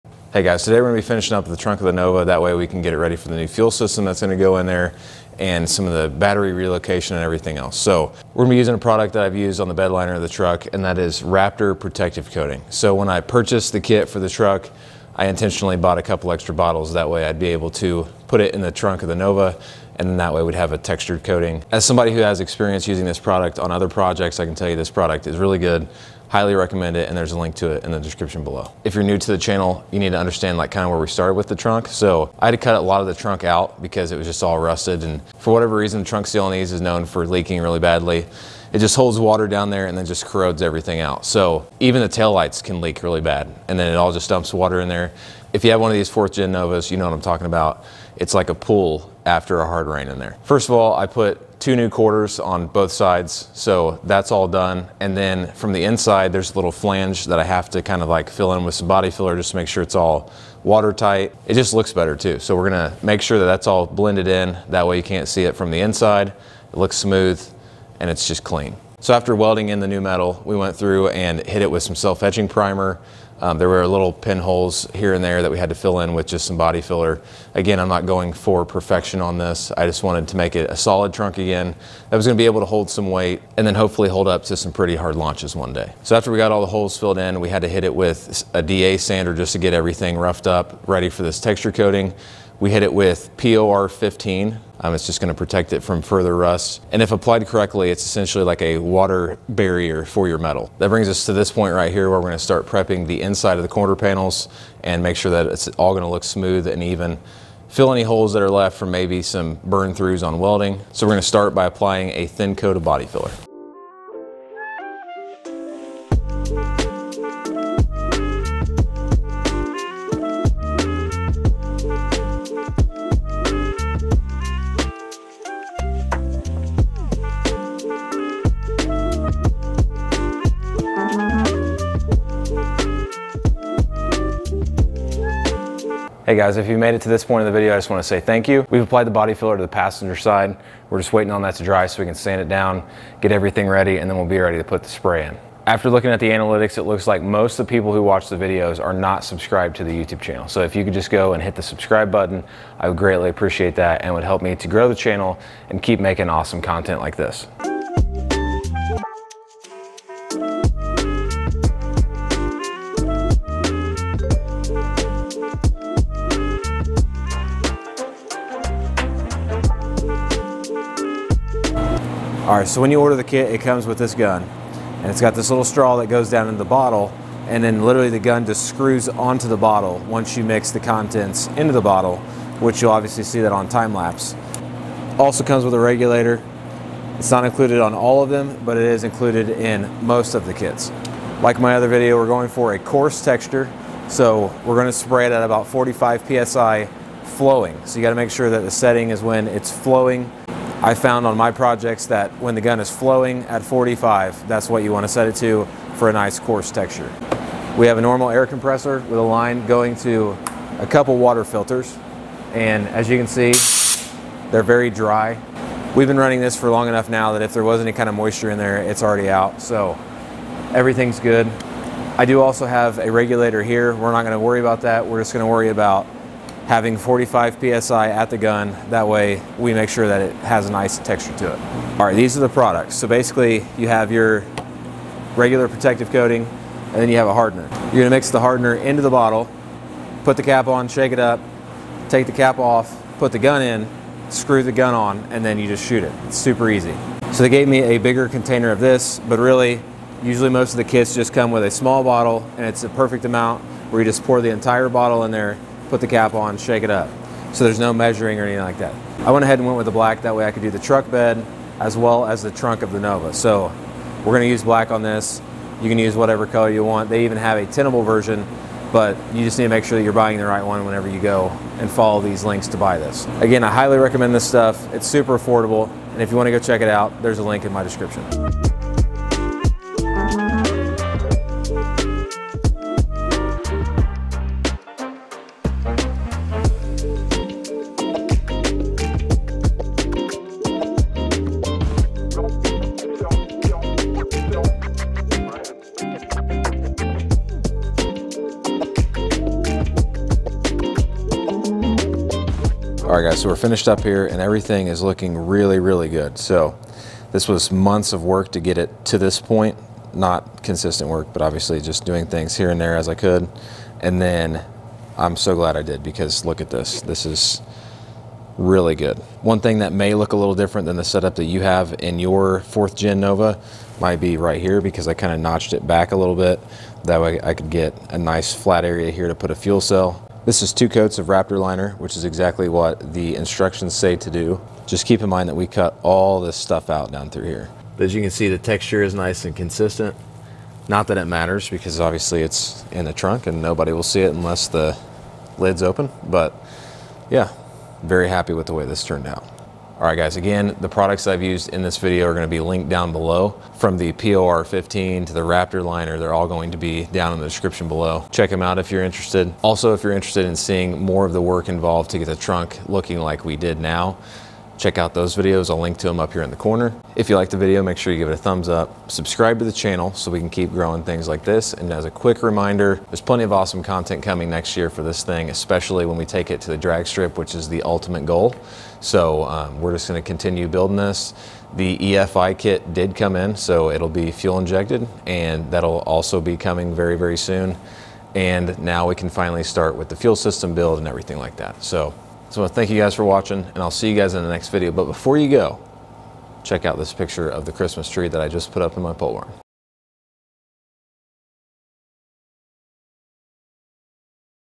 Looks Awesome | 1977 Nova Raptor Liner Trunk
After several months of work, I was able to get my trunk sprayed with bed liner. It turned out great, using @UPOLRaptor Raptor Liner. This liner is a great product, with excellent durability. I recommend this product to anyone!